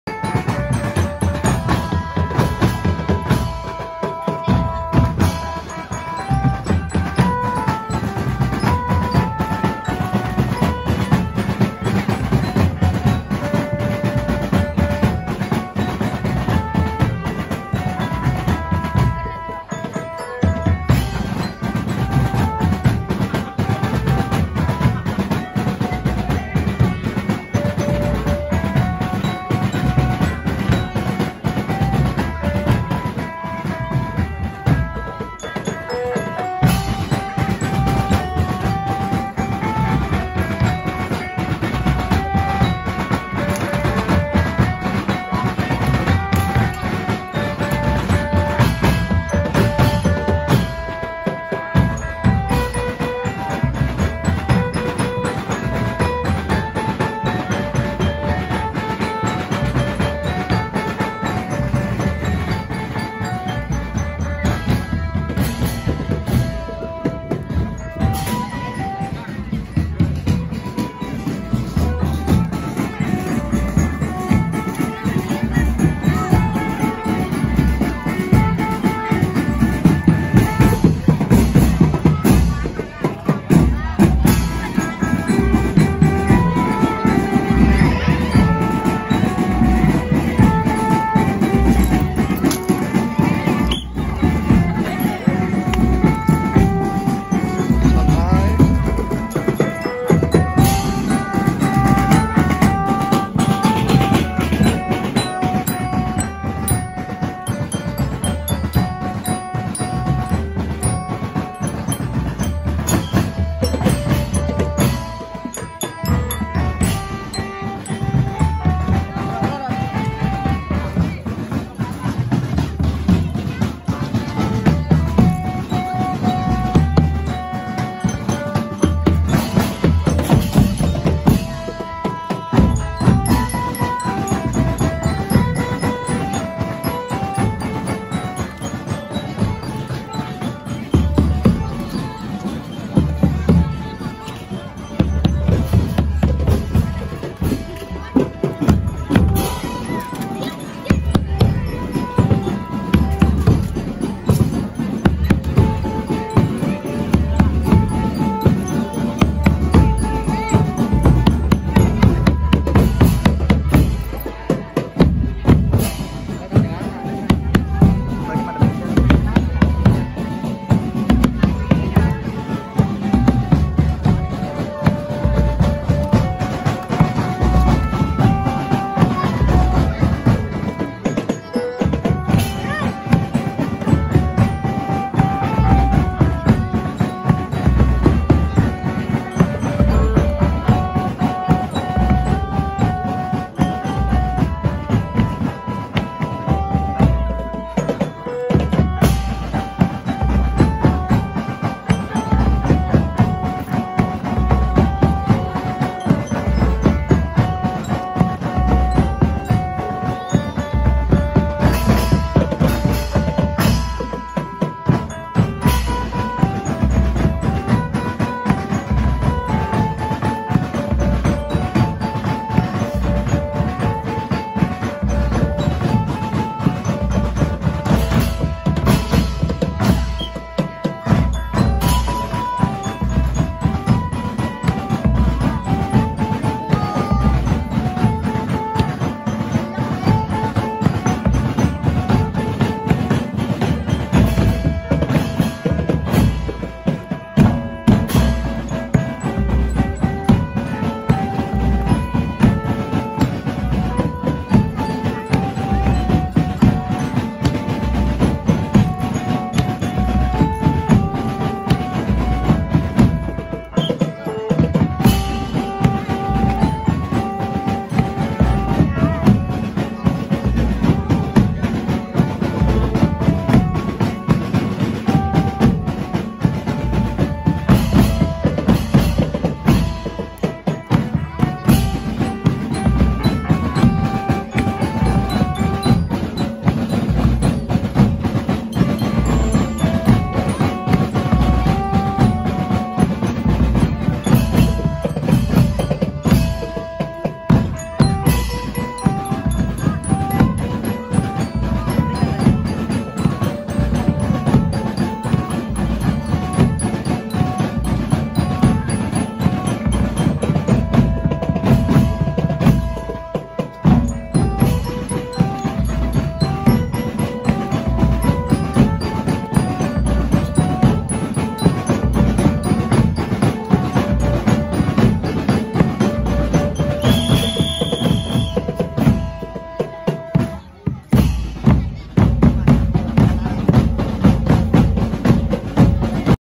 Penampilan Gema At taqwa Marchingband dari MTs At taqwa Jatingarang
Penampilan Gema At taqwa Marchingband di Desa Asemdoyong Kecamatan Taman Kabupaten Pemalang